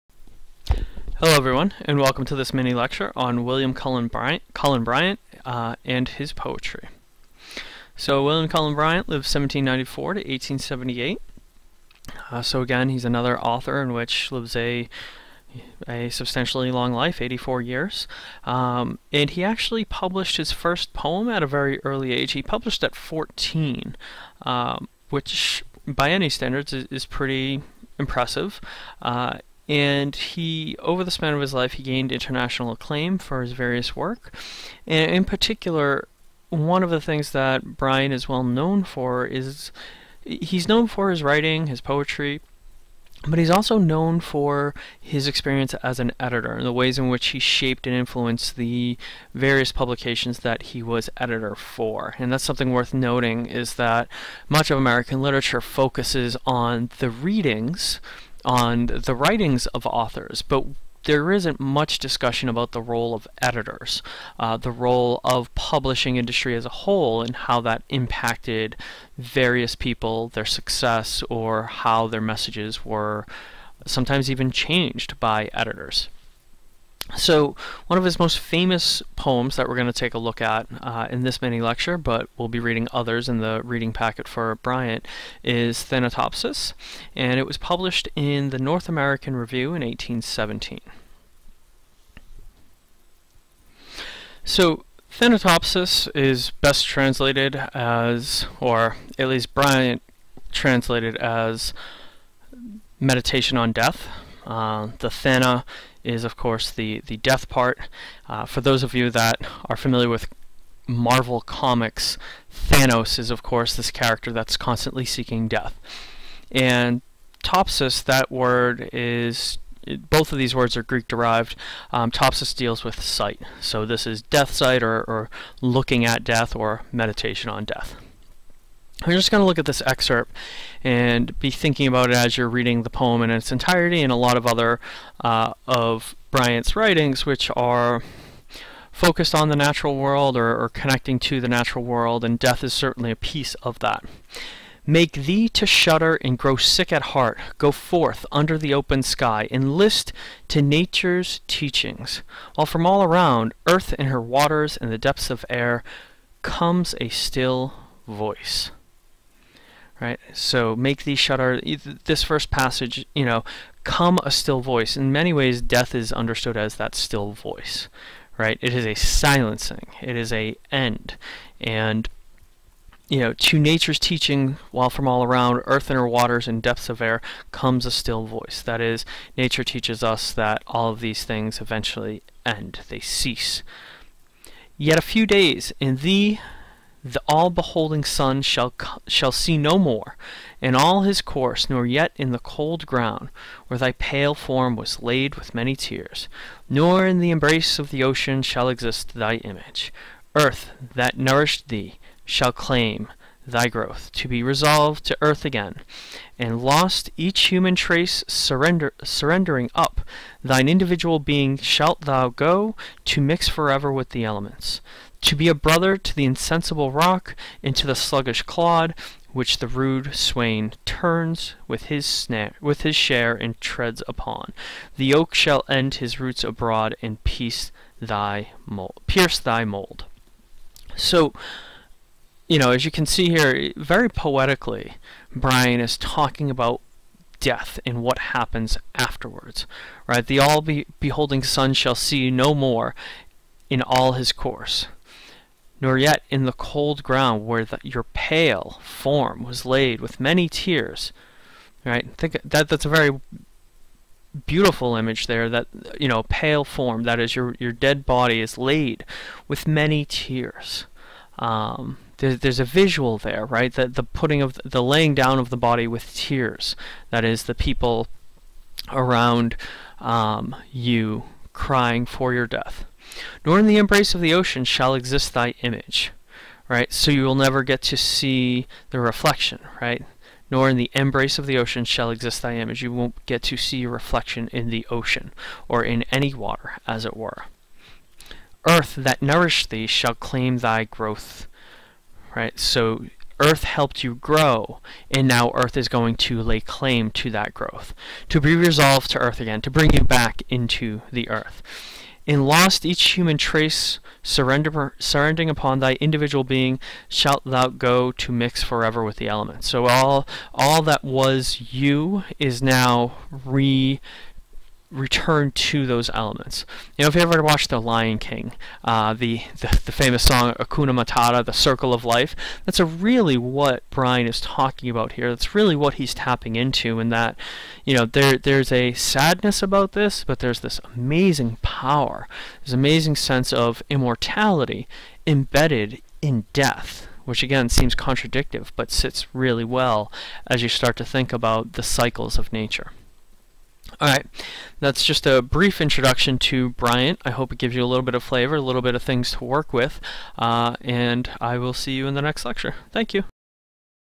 William Cullent Bryant - Poetry
A brief video introduction to William Cullen Bryant and his poems for an American Literature 1 course taught at North Shore Community College in the hybrid-flexible model by Lance Eaton.